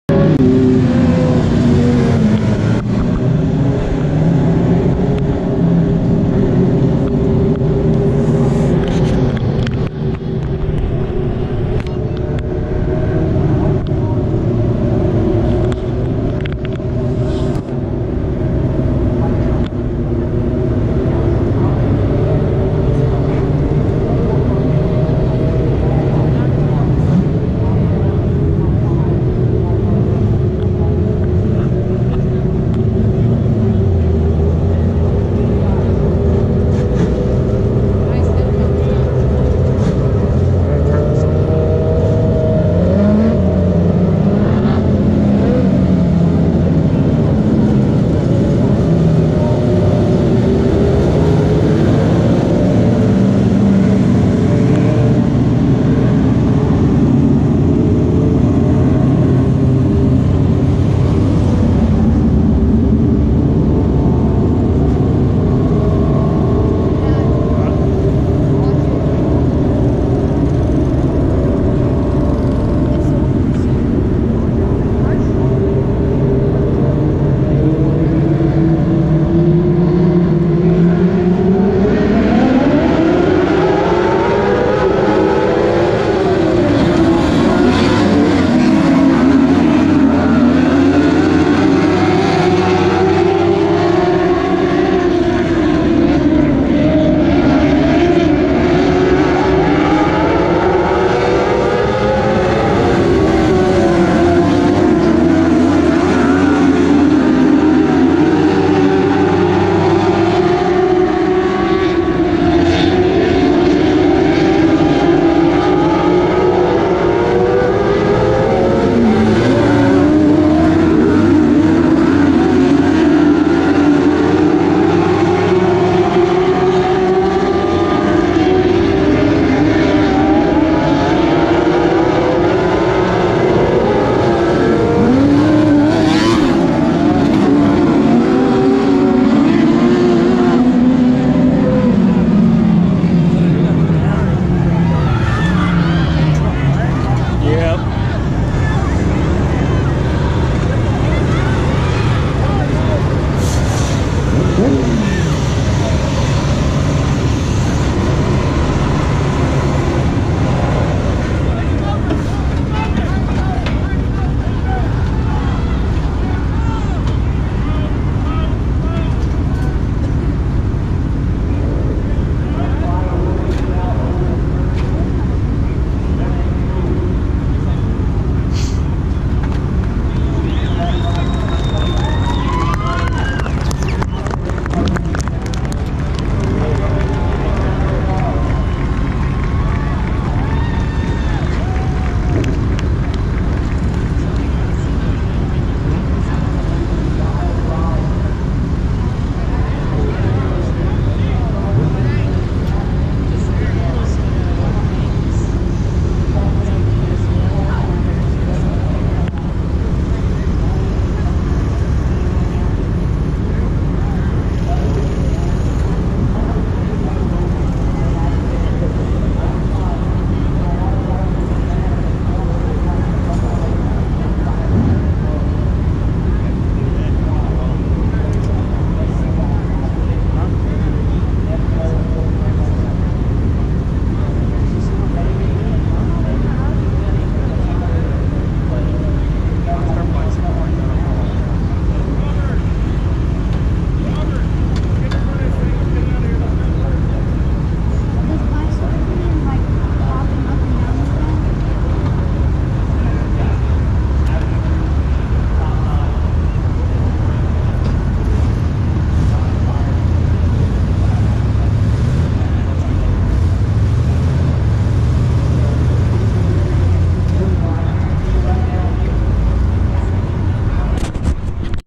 Heart O Texas Speedway fire
Dwarf car Fall Nationals... dwarf car flips and bust into flames...